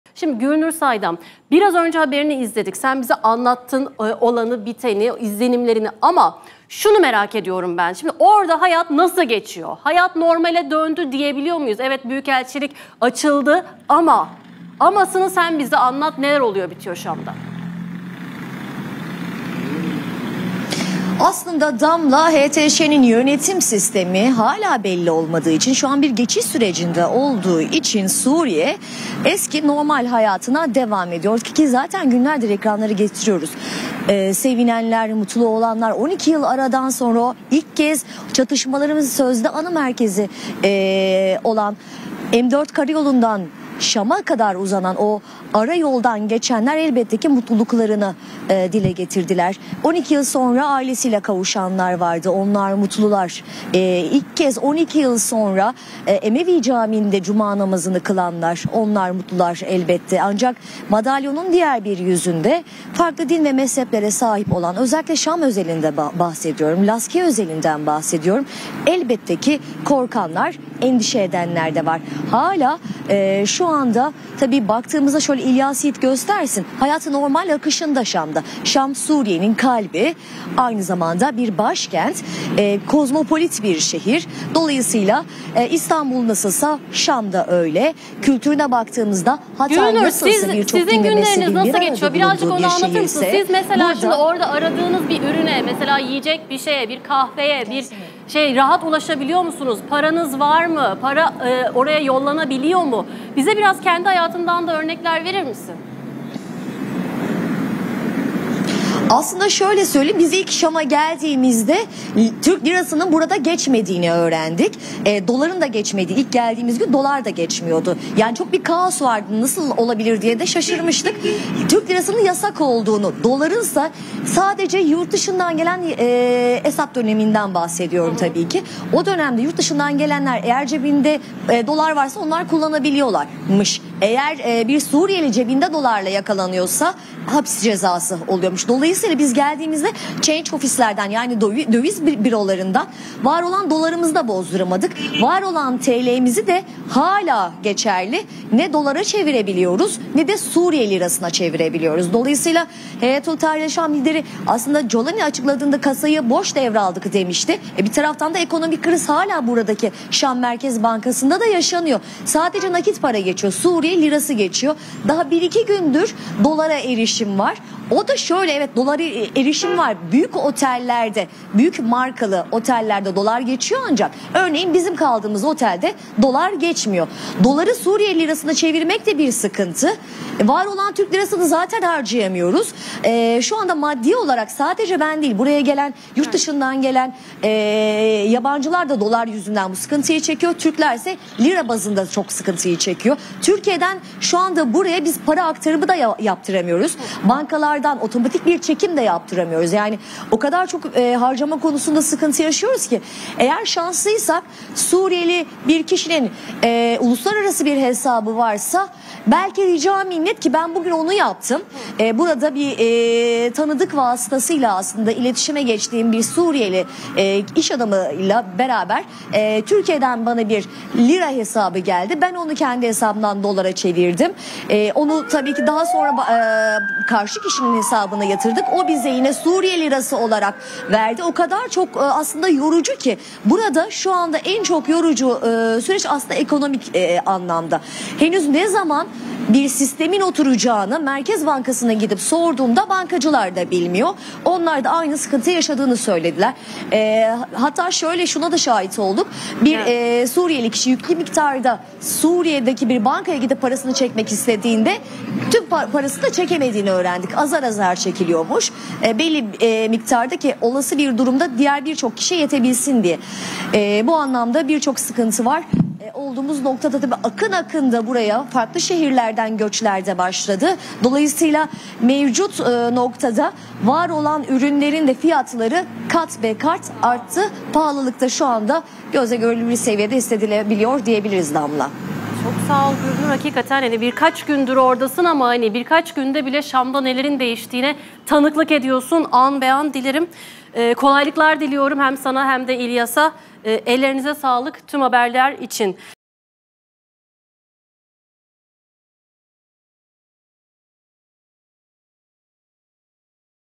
Sözcü TV Suriye Şam'da! Sadece Nakit Para Geçiyor! Dolar ve TL Yasağı! Şoke Olacaksınız
Digiturk: 60
D-smart: 93
Turkcell TV Plus: 69
Tivibu: 69
Türksat Kablo TV: 76